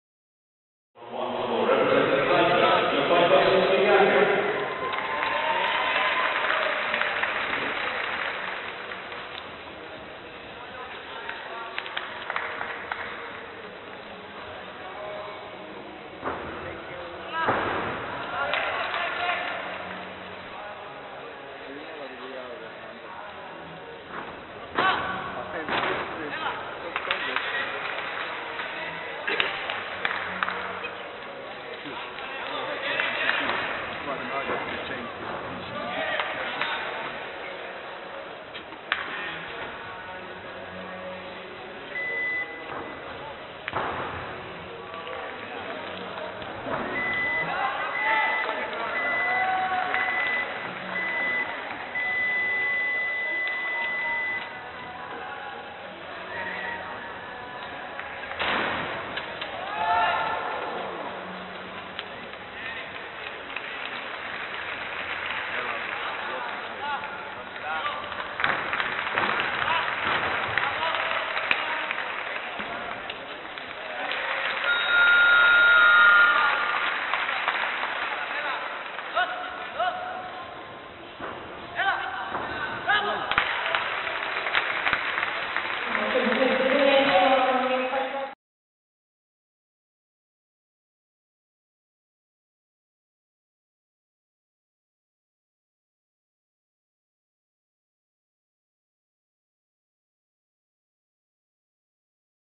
Neofytos KYRIAKOU (CYP) - 2018 Artistic Gymnastics Europeans, qualification floor
Neofytos KYRIAKOU (CYP) scored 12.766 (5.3) on floor in the qualification round of Glasgow 2018.